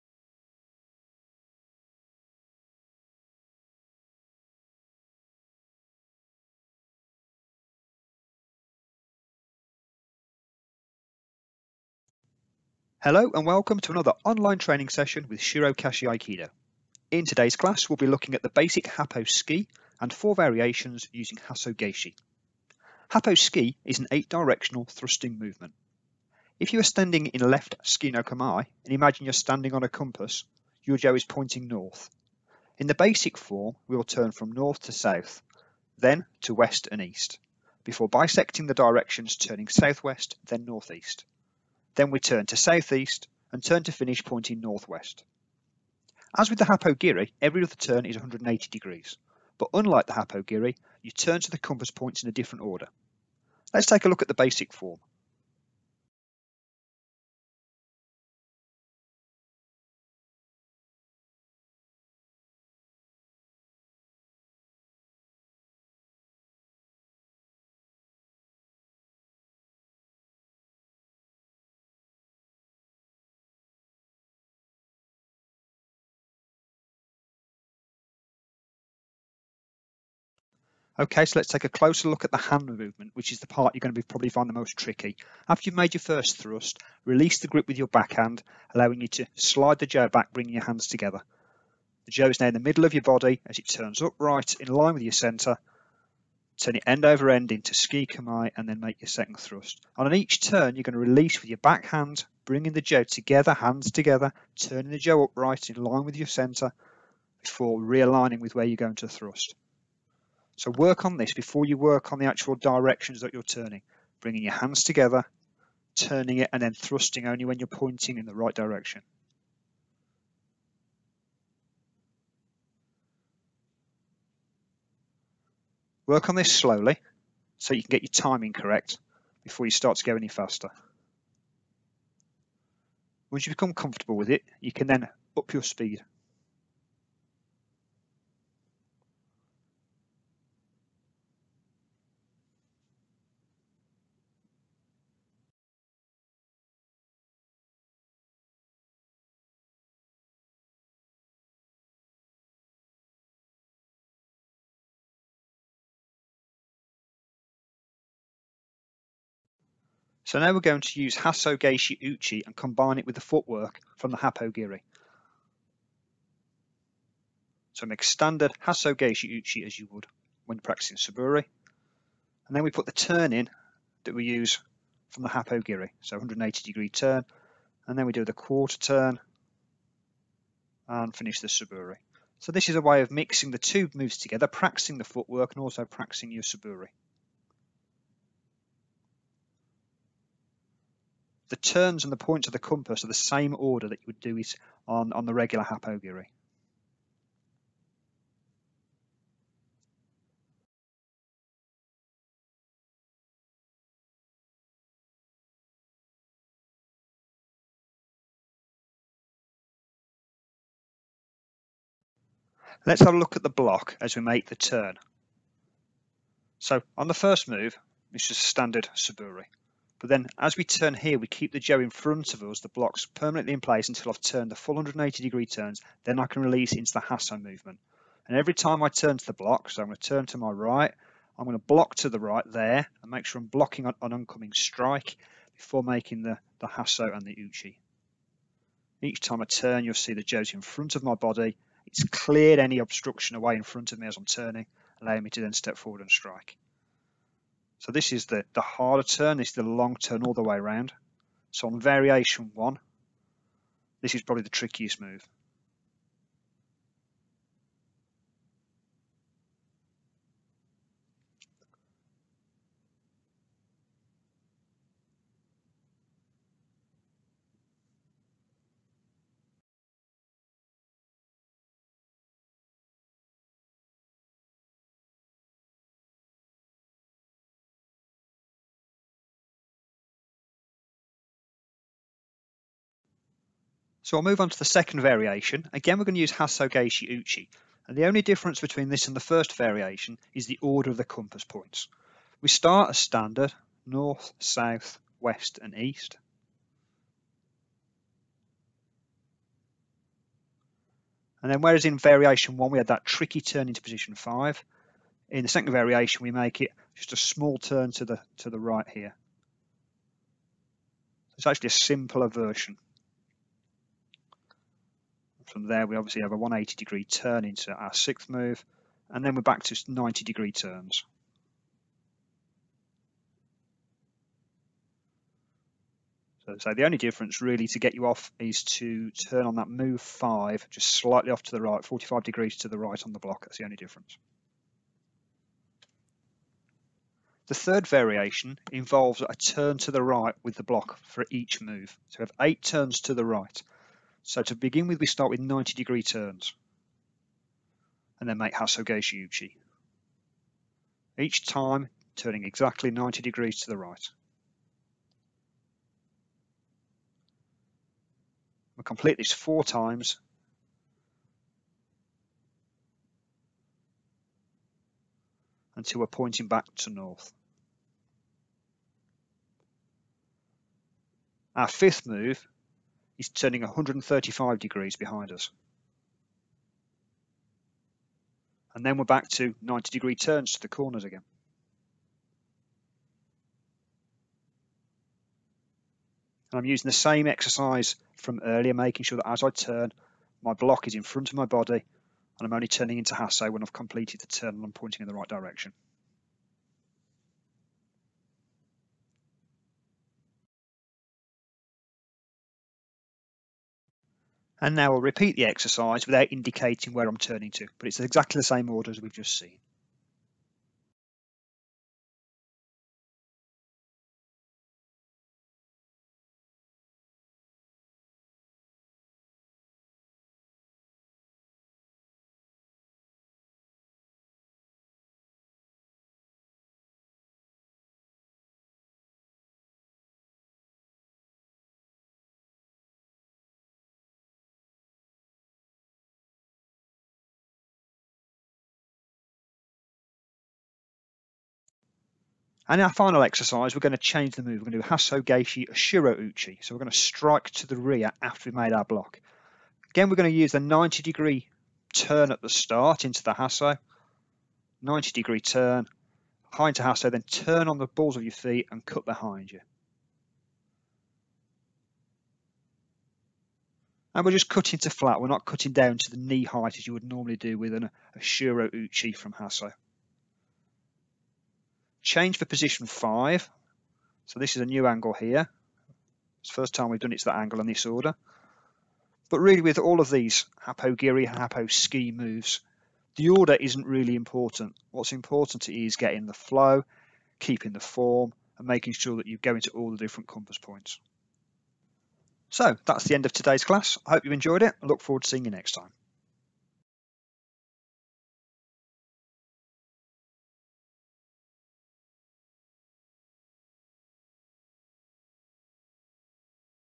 Aikido Weapons: Happo Tsuki & Variations | Shiro Kashi Aikido
In this video, Sensei David Law - 5th Dan - head instructor of Shiro Kashi Aikido focuses on the Happo Tsuki, the eight directional thrusting movement. As well as explaining the standard form, he demonstrates additional exercises incorporating movements from the Hasso Gaeshi series.